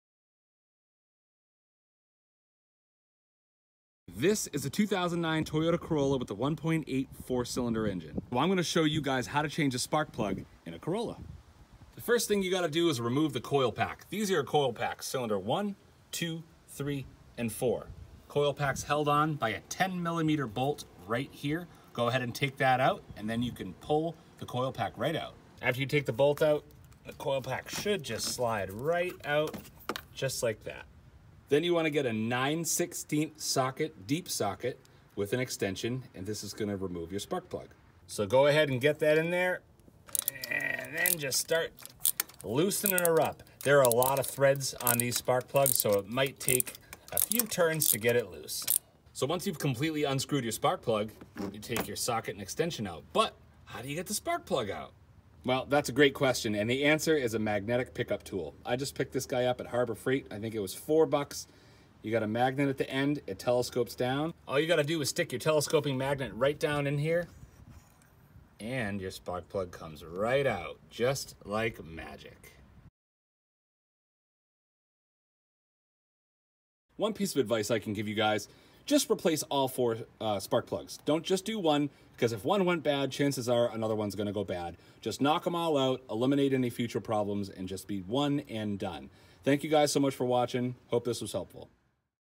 Toyota Corolla 1.8 Spark Plug Change - Simple Instruction
Here's how to change your spark plugs on a Toyota 1.8 engine!